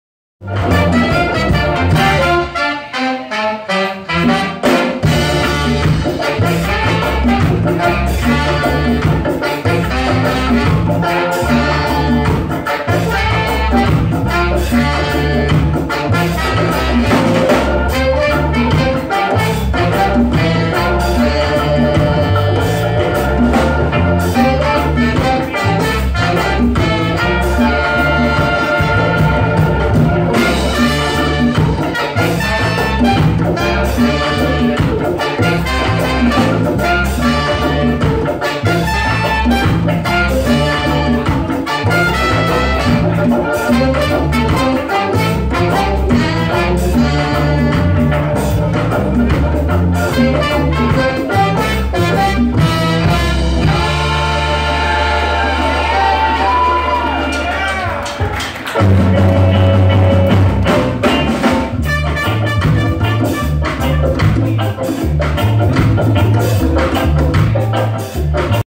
BUDDY JAY'S JAMAICAN JAZZ BAND at The Highline Seattle
10/06/2017 at The Highline in Capitol Hill.